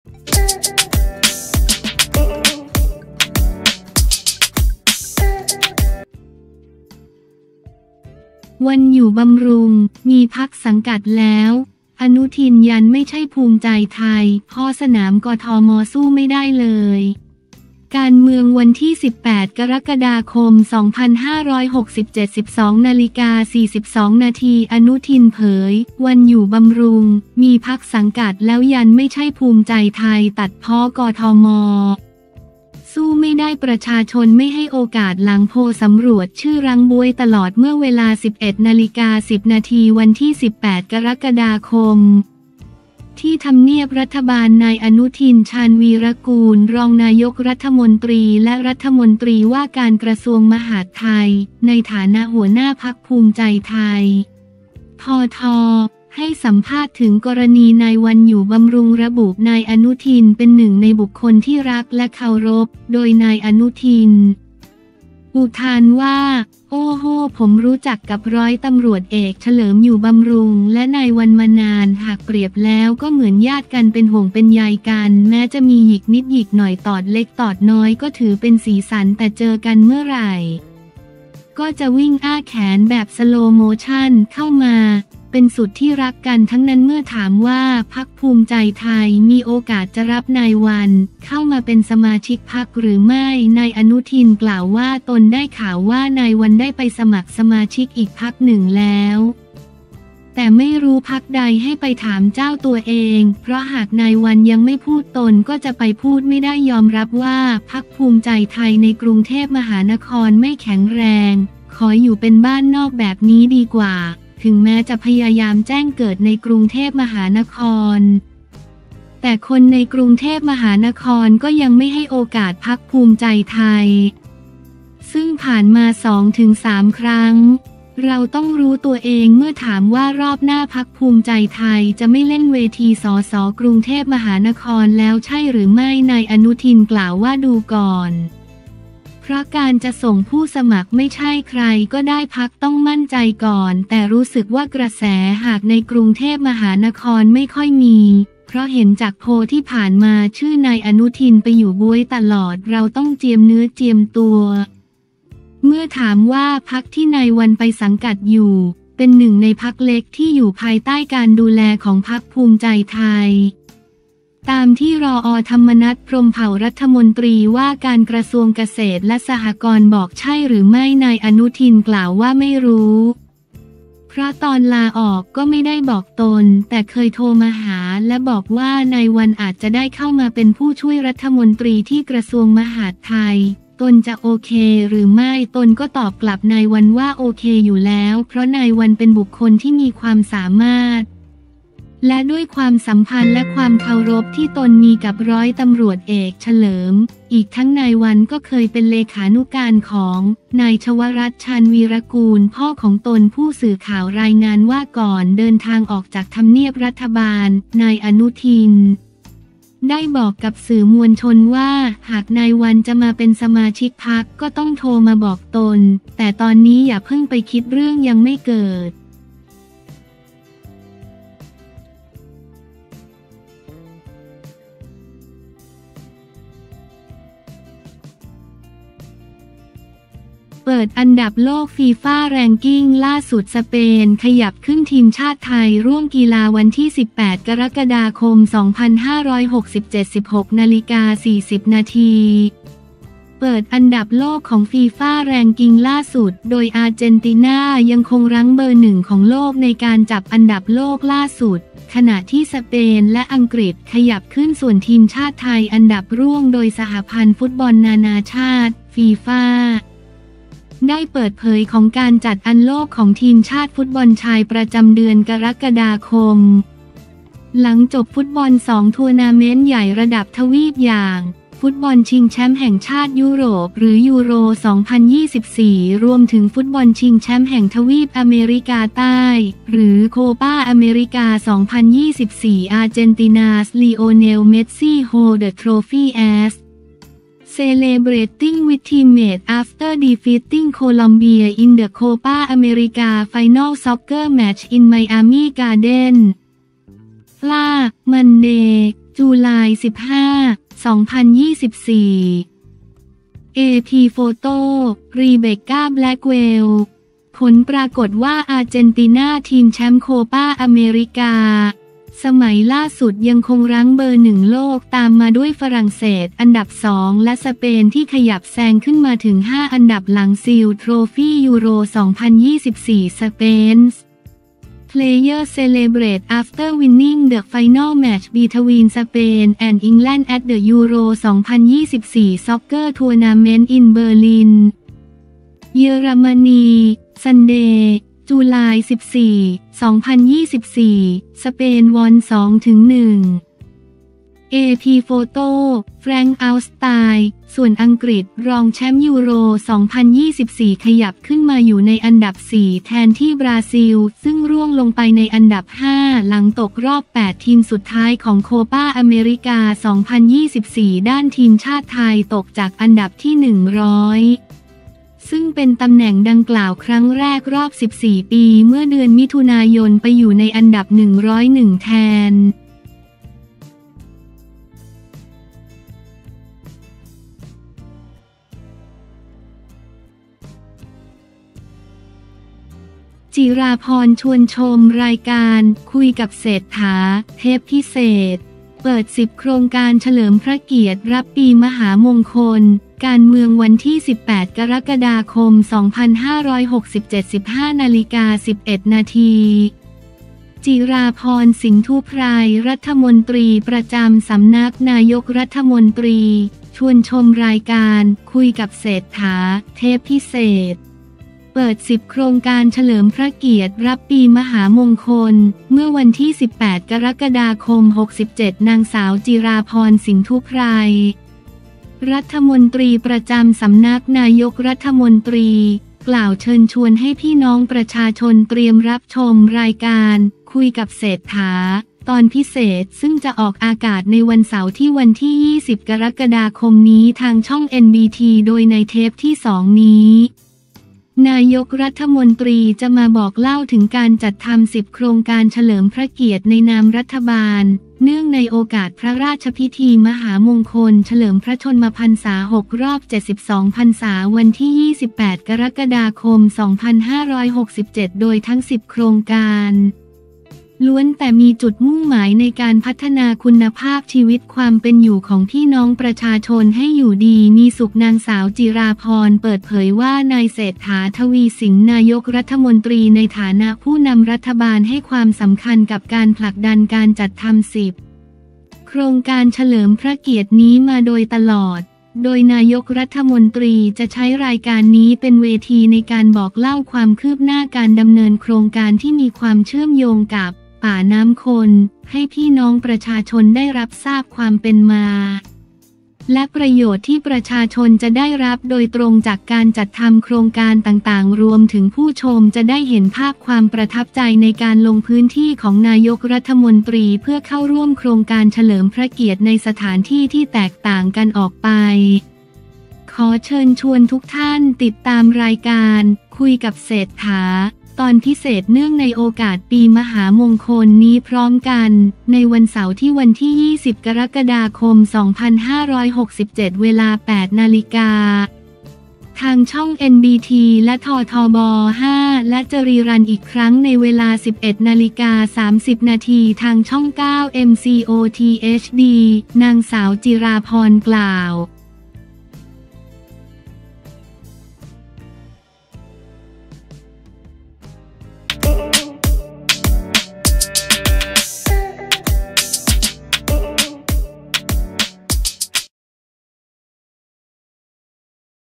‘วัน อยู่บำรุง’ มีพรรคสังกัดแล้ว! อนุทิน ยันไม่ใช่ภูมิใจไทย พ้อสนามกทม สู้ไม่ได้เลย
18 ก.ค. 2567
12:42 น.
อนุทิน เผย ‘วัน อยู่บำรุง’ มีพรรคสังกัดแล้ว ยันไม่ใช่ภูมิใจไทย ตัดพ้อ กทม. สู้ไม่ได้ ประชาชนไม่ให้โอกาส หลังโพลสำรวจชื่อรั้งบ๊วยตลอด

เมื่อเวลา 11.10 น. วันที่ 18 ก.ค. ที่ทำเนียบรัฐบาล นายอนุทิน ชาญวีรกูล รองนายกรัฐมนตรีและรมว.มหาดไทย ในฐานะหัวหน้าพรรคภูมิใจไทย (ภท.) ให้สัมภาษณ์ถึงกรณี นายวัน อยู่บำรุง ระบุนายอนุทินเป็นหนึ่งในบุคคลที่รักและเคารพ โดยนายอนุทิน อุทานว่า “โอ้โห้ ผมรู้จักกับ ร.ต.อ.เฉลิม อยู่บำรุง และนายวัน มานาน หากเปรียบแล้วก็เหมือนญาติกัน เป็นห่วงเป็นใยกัน แม้จะมีหยิกนิดหยิกหน่อย ตอดเล็กตอดน้อย ก็ถือเป็นสีสัน แต่เจอกันเมื่อไหร่ ก็จะวิ่งอ้าแขนแบบสโลว์โมชั่นเข้ามา เป็นสุดที่รักกันทั้งนั้น
เมื่อถามว่าพรรคภูมิใจไทยมีโอกาสจะรับนายวัน เข้ามาเป็นสมาชิกพรรคหรือไม่ นายอนุทินกล่าวว่า ตนได้ข่าวว่านายวัน ได้ไปสมัครสมาชิกอีกพรรคหนึ่งแล้ว แต่ไม่รู้พรรคใด ให้ไปถามเจ้าตัวเอง เพราะหาก นายวัน ยังไม่พูดตนก็จะไปพูดไม่ได้
ยอมรับว่าพรรคภูมิใจไทยในกรุงเทพฯไม่แข็งแรง ขออยู่เป็นบ้านนอกแบบนี้ดีกว่า ถึงแม้จะพยายามแจ้งเกิดในกรุงเทพฯ แต่คนในกรุงเทพฯก็ยังไม่ให้โอกาสพรรคภูมิใจไทย ซึ่งผ่านมา 2-3 ครั้ง เราต้องรู้ตัวเอง

เมื่อถามว่ารอบหน้าพรรคภูมิใจไทยจะไม่เล่นเวที สส.กรุงเทพฯแล้วใช่หรือไม่ นายอนุทิน กล่าวว่า ดูก่อน เพราะการจะส่งผู้สมัคร ไม่ใช่ใครก็ได้ พรรคต้องมั่นใจก่อน แต่รู้สึกว่ากระแสหากในกรุงเทพฯไม่ค่อยมี เพราะเห็นจากโพลที่ผ่านมา ชื่อนายอนุทินไปอยู่บ๊วยตลอด เราต้องเจียมเนื้อเจียมตัว
เมื่อถามว่าพรรคที่นายวันไปสังกัดอยู่ เป็นหนึ่งในพรรคเล็กที่อยู่ภายใต้การดูแลของพรรคภูมิใจไทย ตามที่ร.อ.ธรรมนัส พรหมเผ่า รมว.เกษตรและสหกรณ์ บอกใช่หรือไม่ นายอนุทินกล่าวว่า ไม่รู้ เพราะตอนลาออกก็ไม่ได้บอกตน

แต่เคยโทรมาหาและบอกว่า นายวันอาจจะได้เข้ามาเป็นผู้ช่วยรัฐมนตรีที่กระทรวงมหาดไทย ตนจะโอเคหรือไม่ ตนก็ตอบกลับนายวันว่า โอเคอยู่แล้ว เพราะนายวันเป็นบุคคลที่มีความสามารถ และด้วยความสัมพันธ์และความเคารพที่ตนมีกับร.ต.อ.เฉลิม อีกทั้งนายวันก็เคยเป็นเลขานุการของ นายชวรัตน์ ชาญวีรกูล พ่อของตน
ผู้สื่อข่าวรายงานว่า ก่อนเดินทางออกจากทำเนียบรัฐบาล นายอนุทิน ได้บอกกับสื่อมวลชนว่า หากนายวัน จะมาเป็นสมาชิกพรรคก็ต้องโทรมาบอกตน แต่ตอนนี้อย่าเพิ่งไปคิด เรื่องยังไม่เกิด